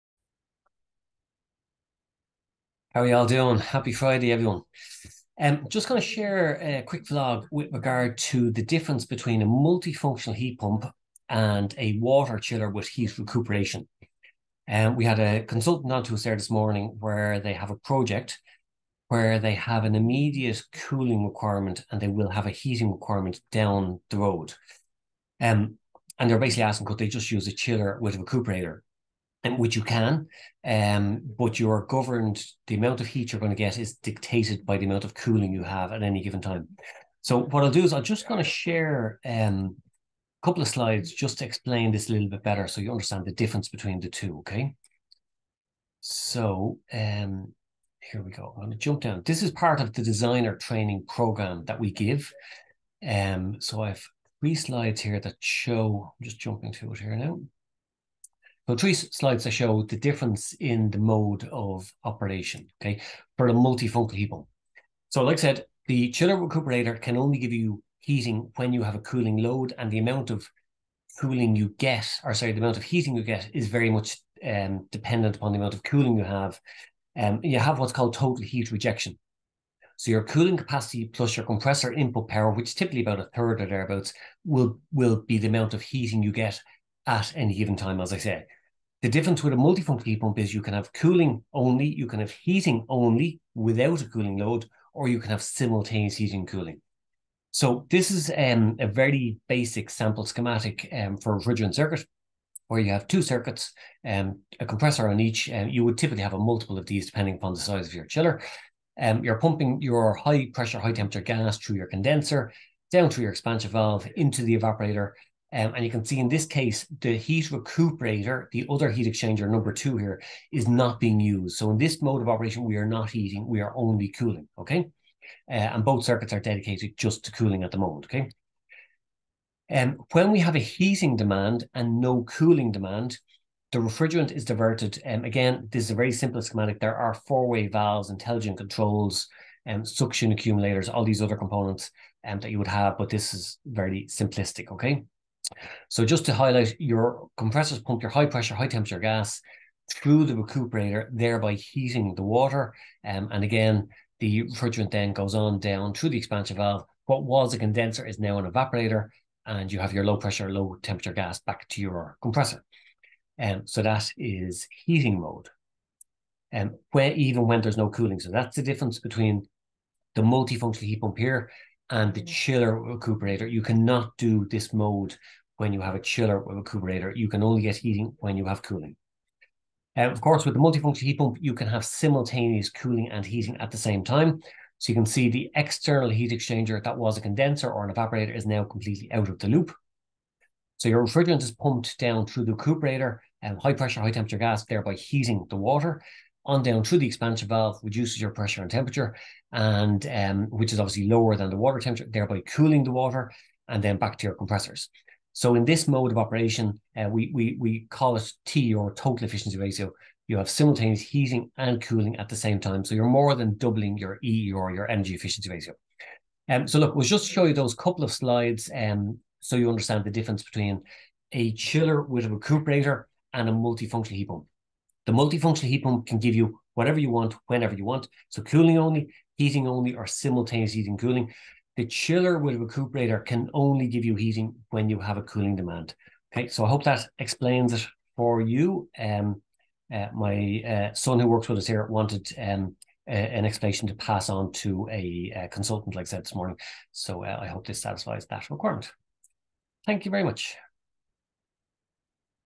Multifunctional Heat Pump v's Chiller with heat recuperator
In this VLOG I explain the difference between a Multifunctional Heat Pump and a Water Chiller with a heat recuperator (or desuperheater).